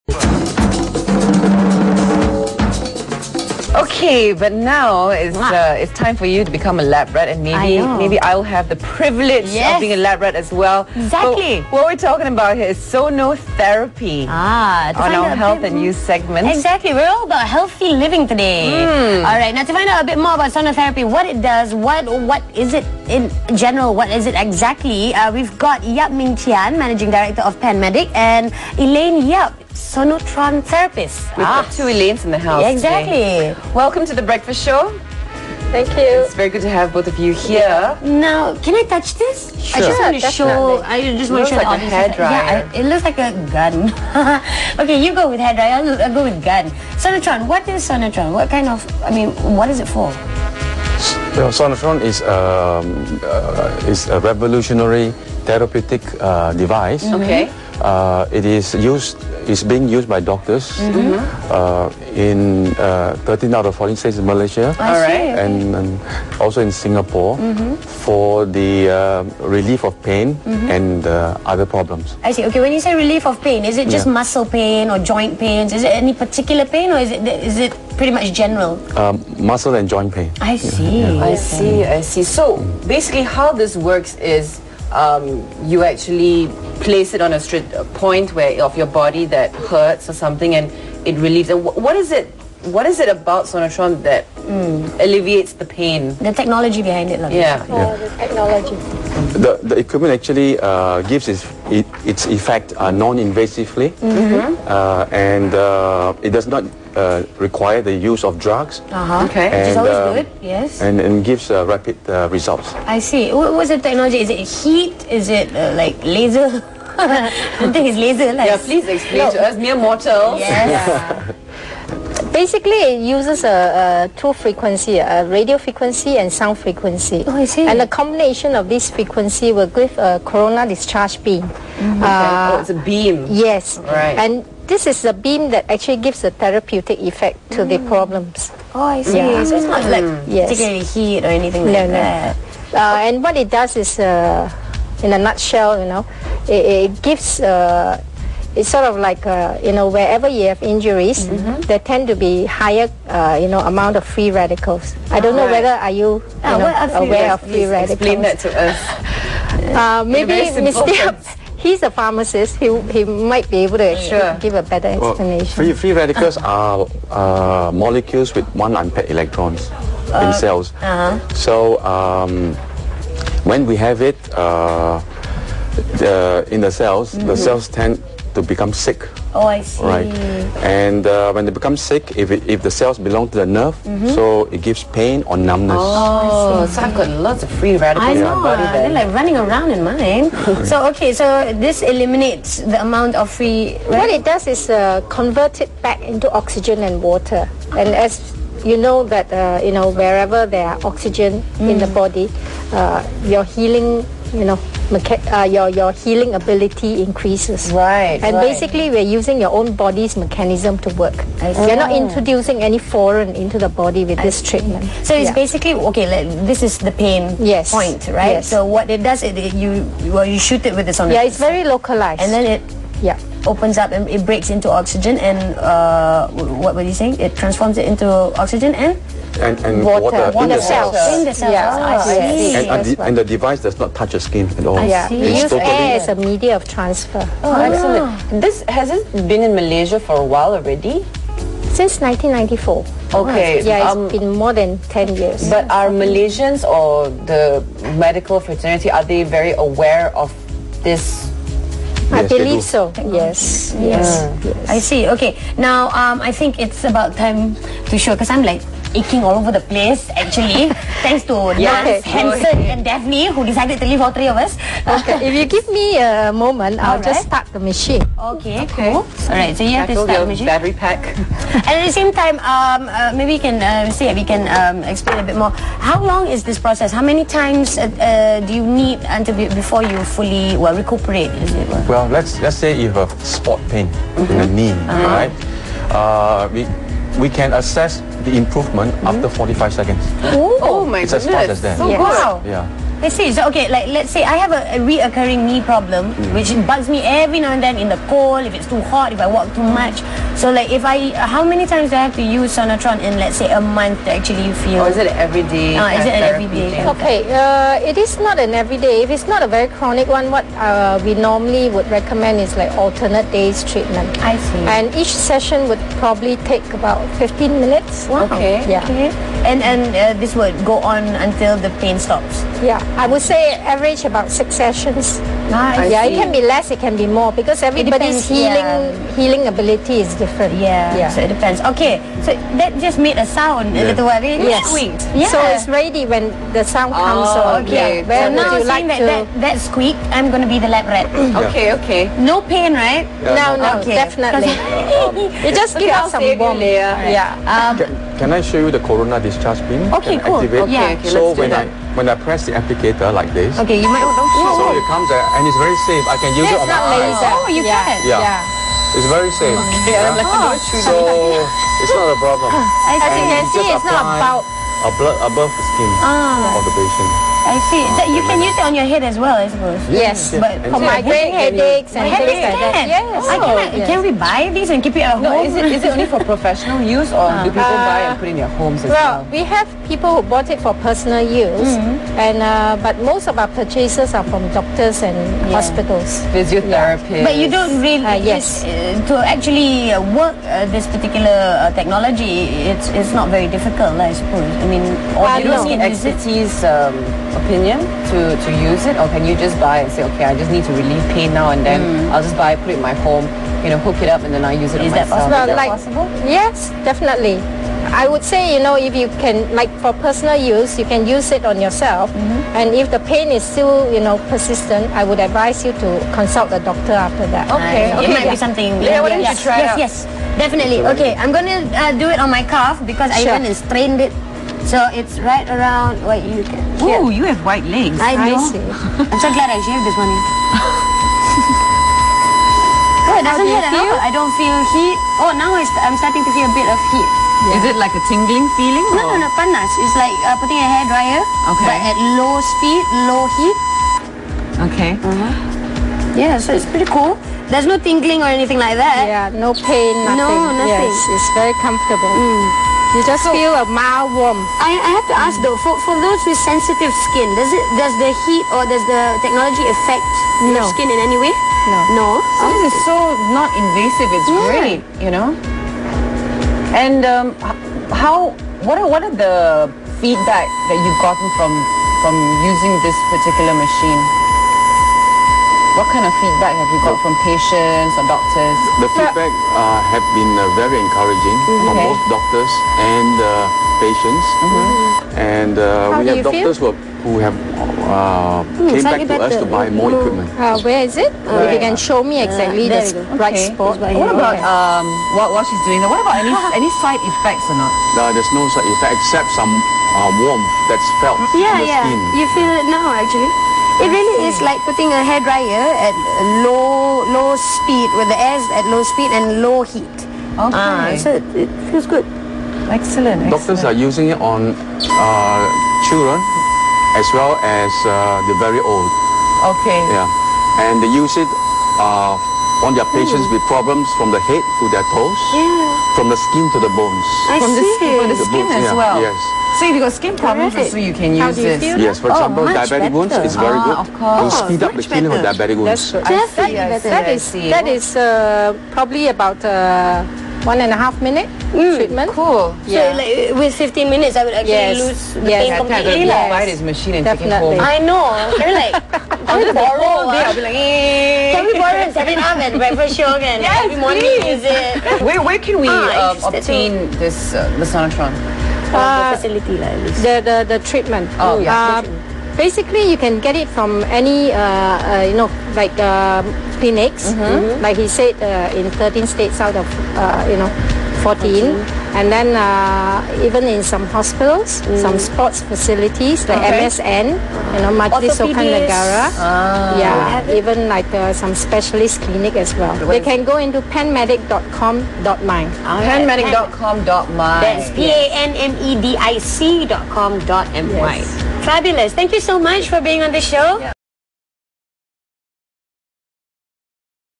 Introduction Of Sonotron On TV
Panmedic introduces Sonotron - a drug-free, rapid, non-invasive, evidence-based therapeutic device for solving chronic and acute clinical problems of patients. If you are new to Sonotron, this TV show will tell you something about the revolutionary, state-of-the-art therapeutic device that delivers its beneficial effects non-invasively, without the need to use drugs, does not give adverse side effects and most importantly delivers excellent patient satisfaction.